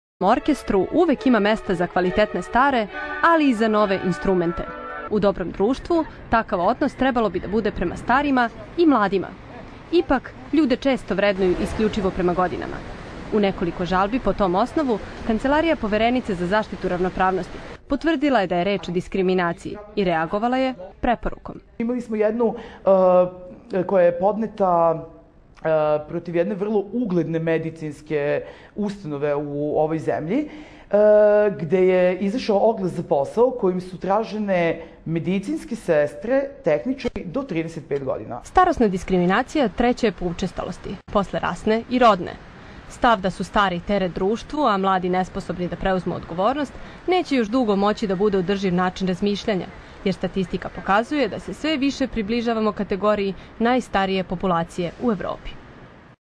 Kosana Beker - Starosna diskriminacija u Srbiji
Pomoćnica poverenice za zaštitu ravnopravnosti, Kosana Beker, govori o učestaloj starosnoj diskriminaciji u Srbiji.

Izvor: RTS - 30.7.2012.